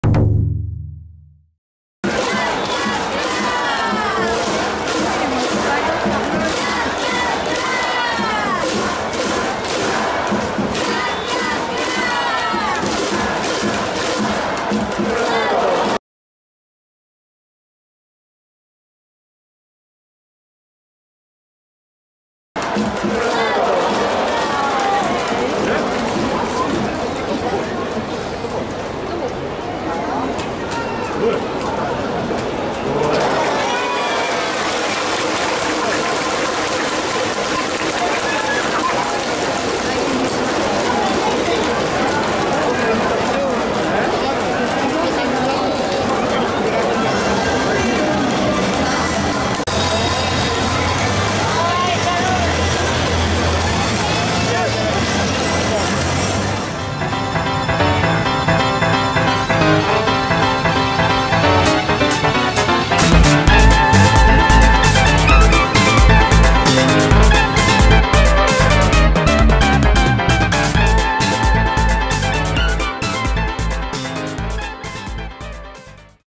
これは…wカープ歴代捕手の伝統を感じさせる坂倉将吾の死球アピールwww#広島#カープ横浜 DeNA ベイスターズハイライト ダイジェスト
8月1日(火)セントラルリーグ
広島東洋カープVS横浜DeNAベイスターズ